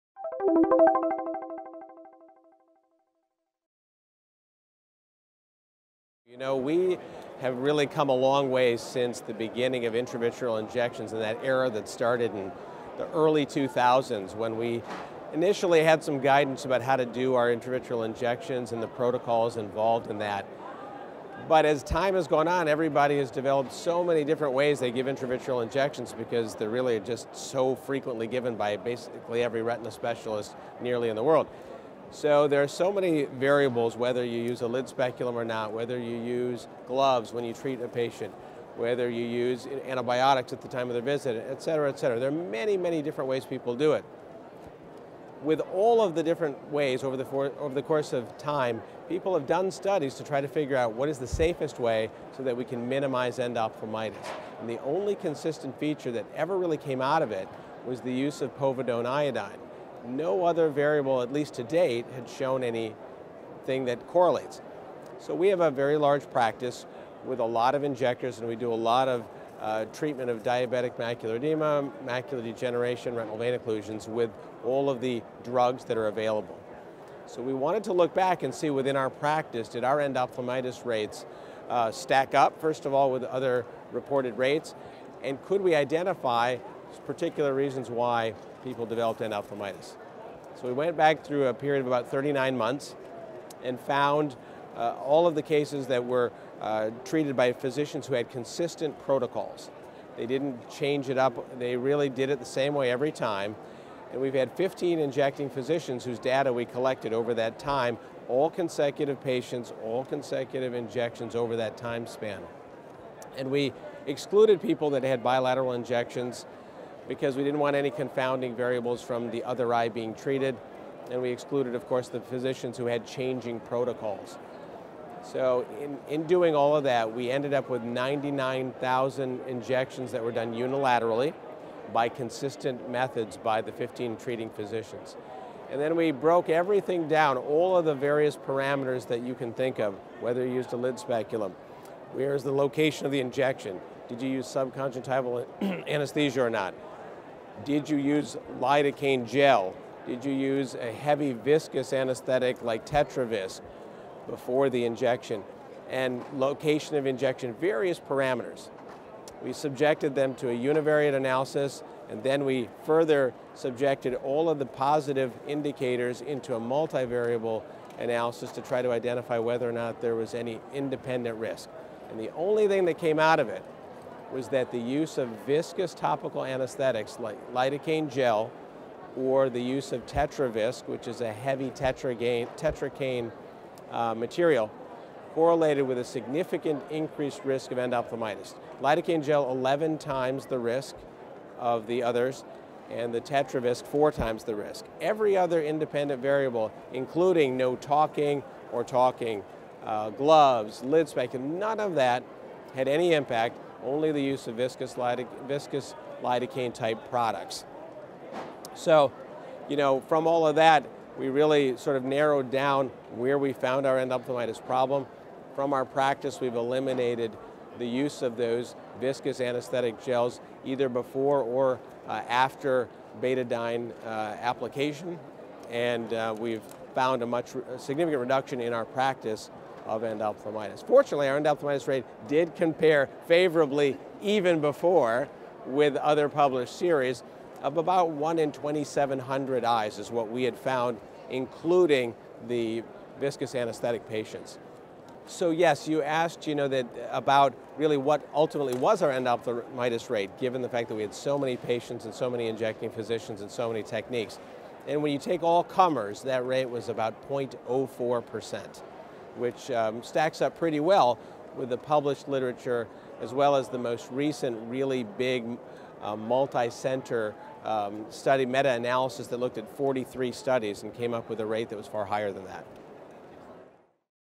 AAO 2019 Retina Street Team: Tarek S. Hassan, MD
Tarek S. Hassan, MD, shares his findings on the predictors of post injection endophthalmitis: a multivariable analysis based on injection protocol and povidone iodine strength, at the 2019 American Academy of Ophthalmology annual meeting.